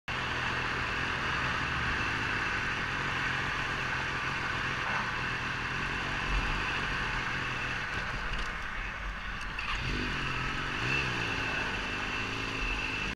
S1000RR dies in traffic.
Mysterious shutdown in traffic part I.  This was about the forth time the bike died, since I got it.  I have about 600 miles on the clock at this point and it was a fairly hot day.  I've only noticed the issue when the bike has heat soaked quite a bit.  On this day, the bike was pretty hot, as the traffic was pretty horrible.